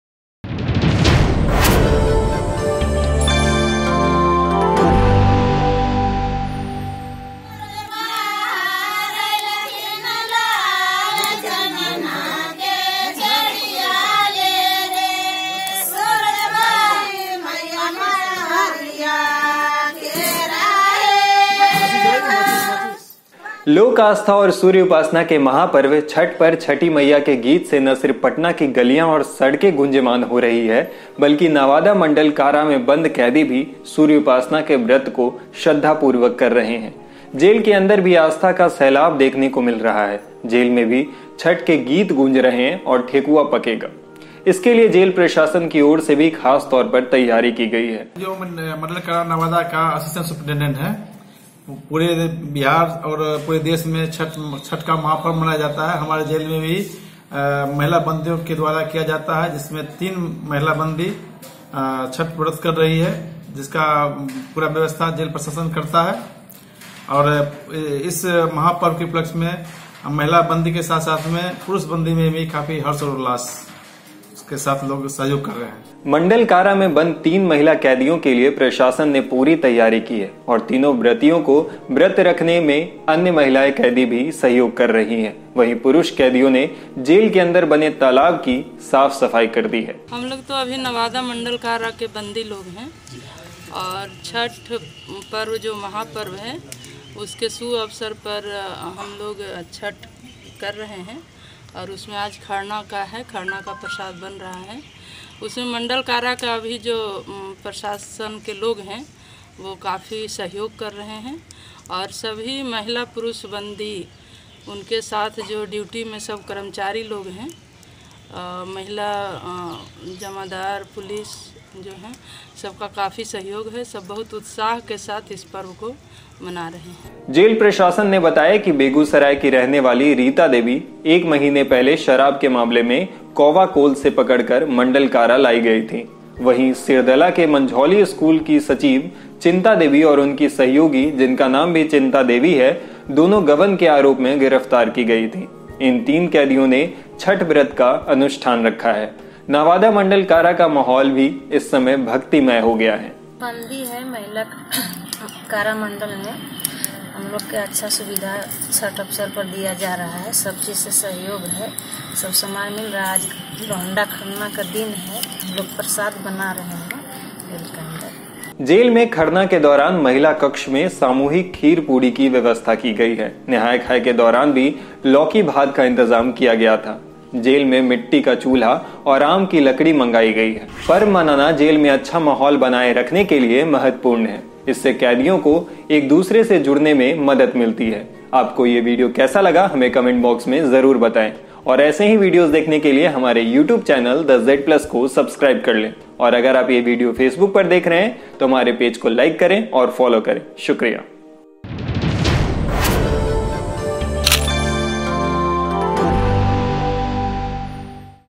Bihar के Jail तक गूंज रहे Chhath के गीत, कैदी भी सूर्योपासना में जुटे, तैयारी में जुटा जेल प्रशासन
लोक आस्था और सूर्य उपासना के महापर्व छठ पर छठी मइया के गीत से न सिर्फ पटना की गलियां और सड़कें गुंजायमान हो रही हैं, बल्कि नवादा मंडल कारा में बंद कैदी भी सूर्योपासना के व्रत को श्रद्धापूर्वक कर रहे हैं. जेल के अंदर भी आस्था का सैलाब देखने को मिल रहा है. जेल में भी छठ के गीत गूंज रहे हैं और ठेकुआ पकेगा. इसके लिए जेल प्रशासन की ओर से भी खासतौर पर तैयारी की गई है. जेल प्रशासन की ओर से मिली जानकारी के मुताबिक, अभी तक तीन महिला बंदियों ने छठ उपासना करने की ठानी है.